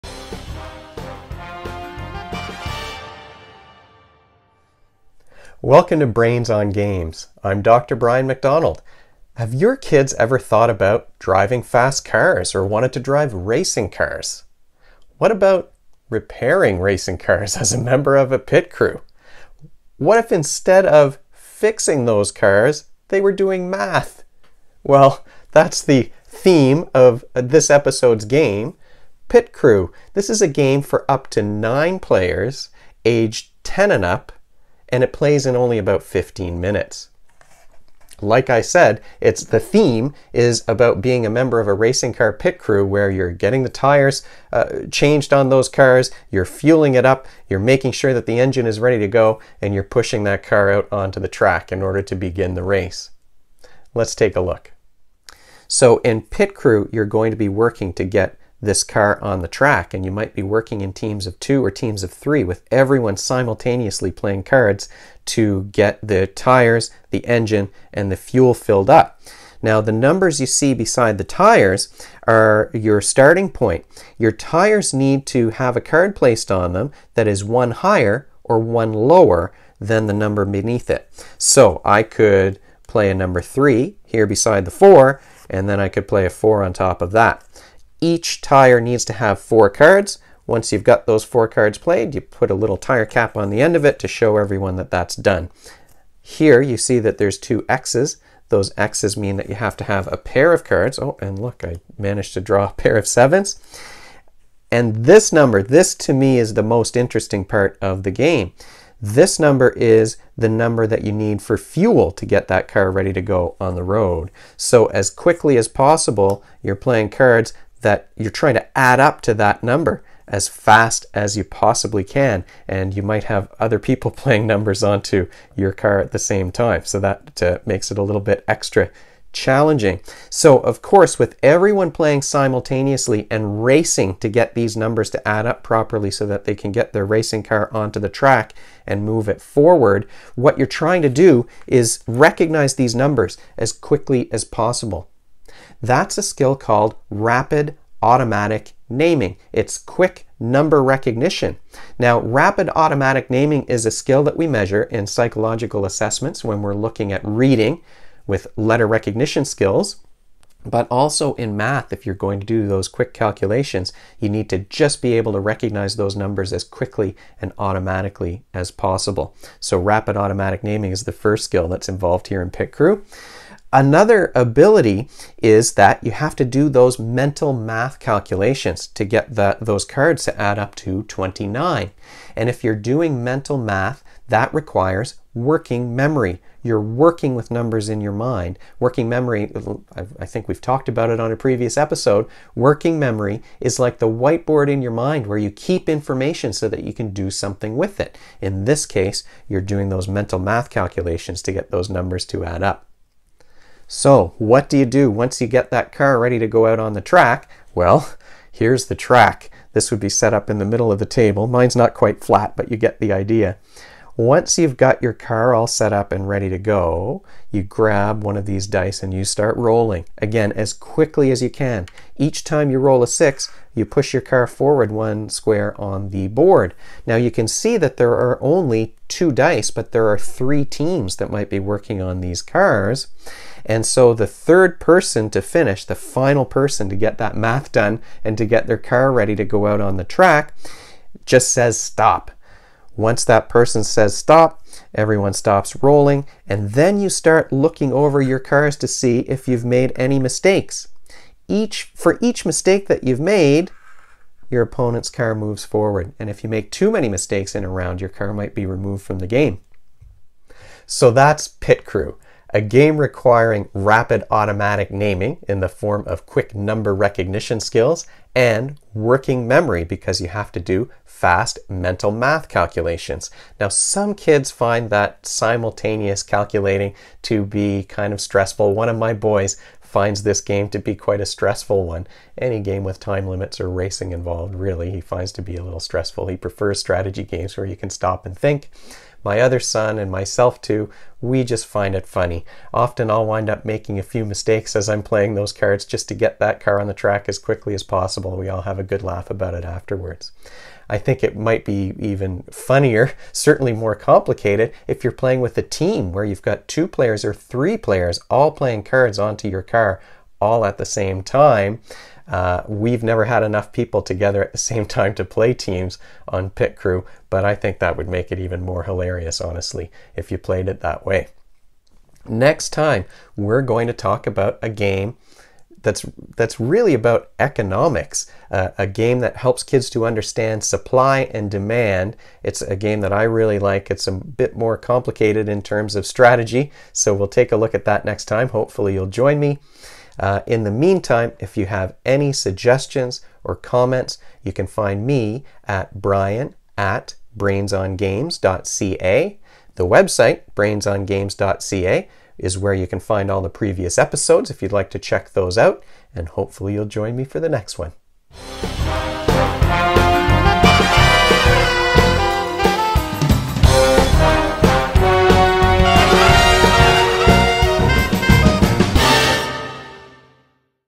Pit Crew
Dr. Brian discusses Pit Crew, a game requiring players to race to complete math calculations.